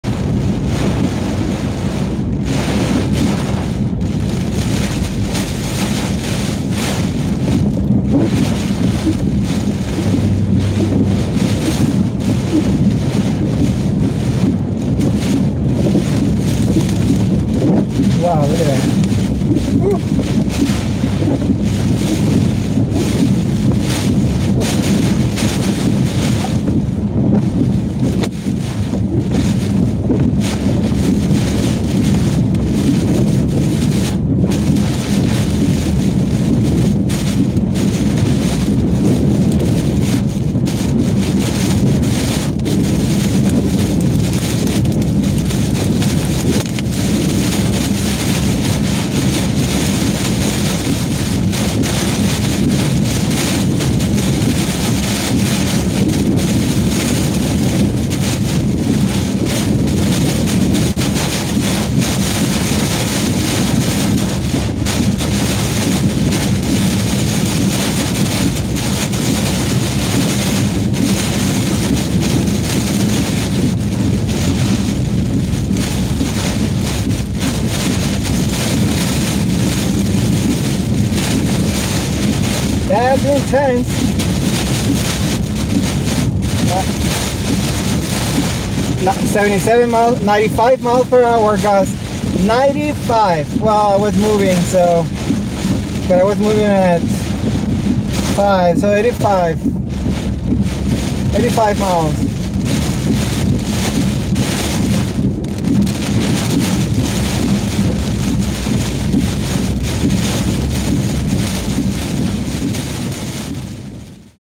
9.18.2022 MESOVORTEX INSIDE THE INNER EYEWAL OF HURRICANE FIONA
DRIVING THROGH A MESOVORTEX INSIDE THE INNER EYEWAL OF HURRICANE FIONA INTENSE HURRICANE FORCE WINDS